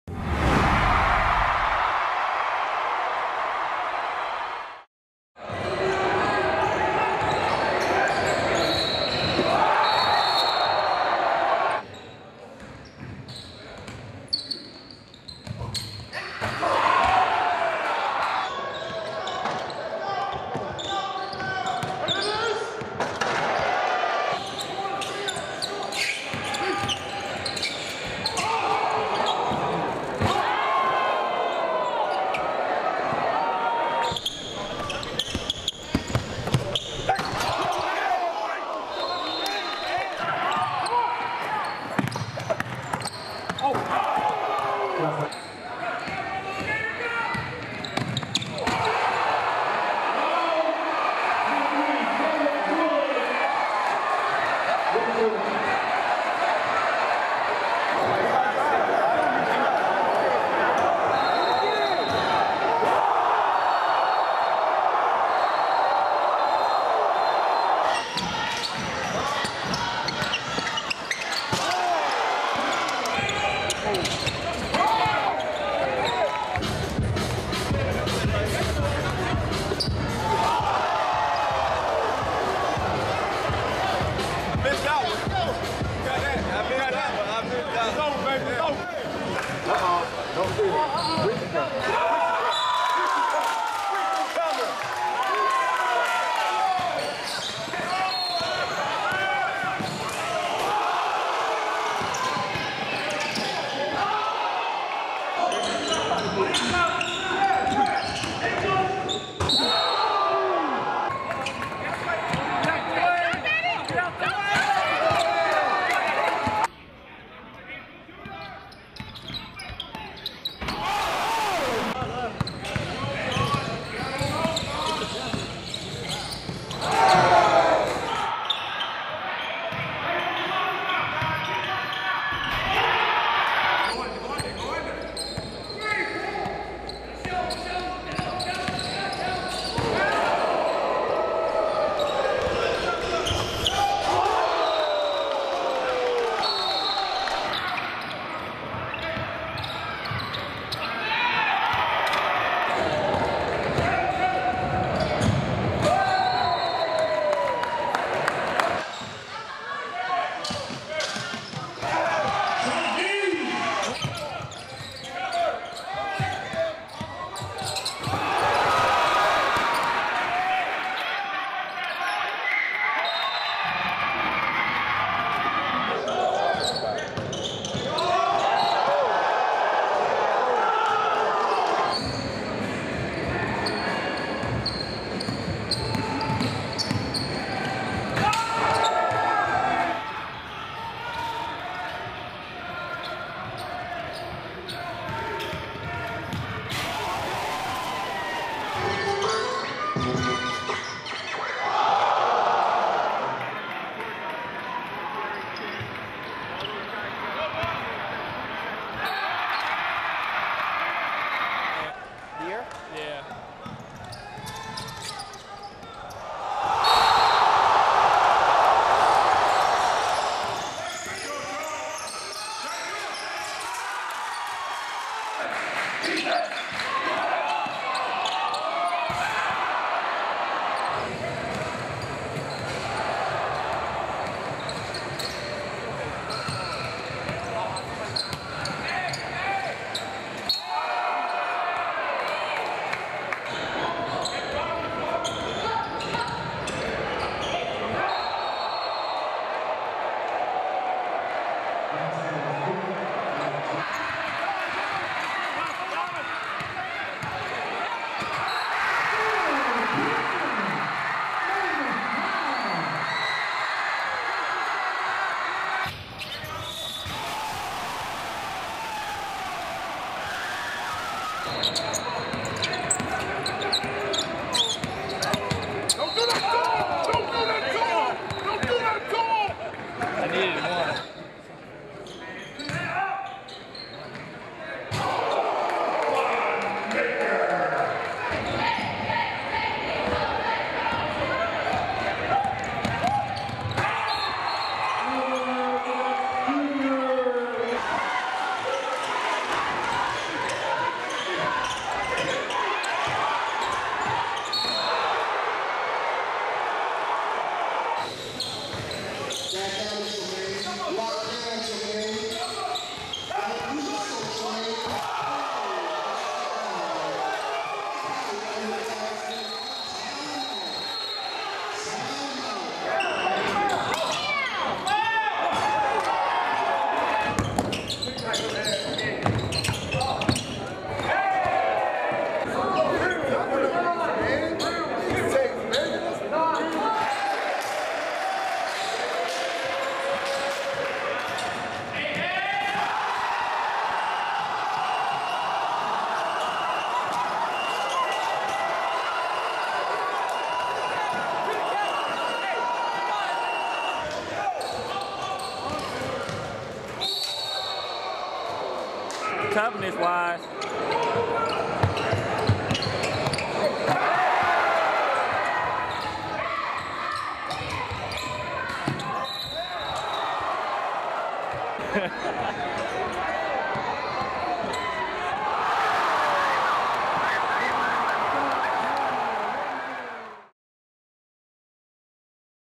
DON'T JUMP Vol. 1! The Best of Elite Mixtapes POSTER Dunks
The best poster dunks in the Elite Mixtapes archive.

Special Thanks to Ballislife.com!

Follow @EliteMixtapes on IG